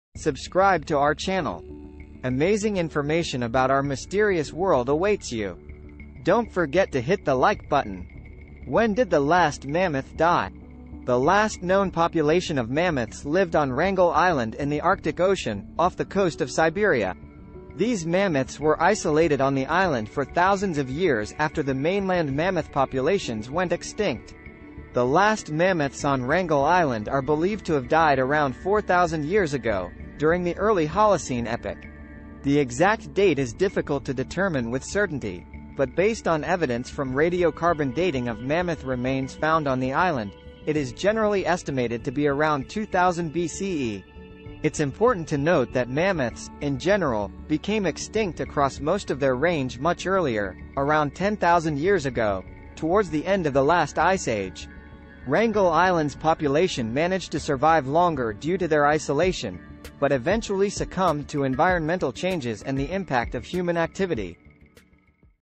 When did the last mammoth die?
In this captivating and informative video, we delve deep into the mysteries surrounding the last mammoth to roam the Earth. Join us on a journey through time as we explore the timeline of mammoth extinction and shed light on the enigmatic tale of the last of these majestic creatures.

Supporting channel development:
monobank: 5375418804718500
Skrill: